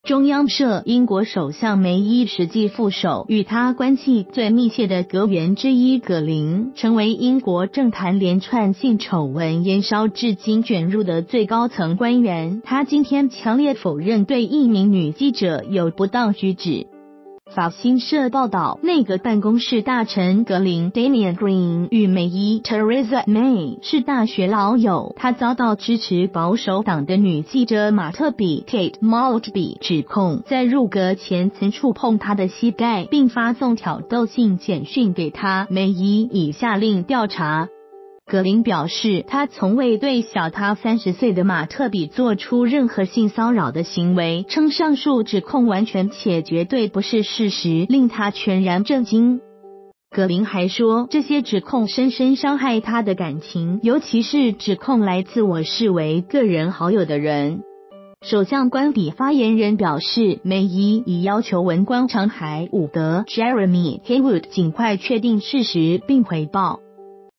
英政壇性騷醜聞延燒 梅伊副手否認指控
〔中央社〕英國首相梅伊實際副手、與她關係最密切的閣員之一葛林，成為英國政壇連串性醜聞延燒至今捲入的最高層官員。他今天強烈否認對一名女記者有不當舉止。

發表評論評論如下。